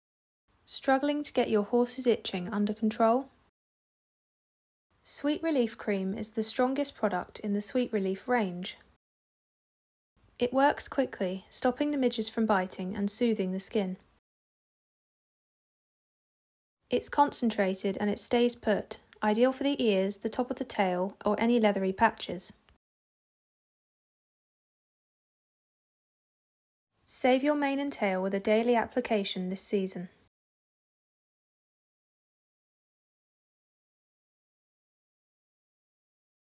Struggling to get your horse’s itching under control?
Sweet Relief cream is the strongest product in the Sweet Relief range
It works quickly, stopping the midges biting and soothing the skin.
Save your mane and tail with a daily application.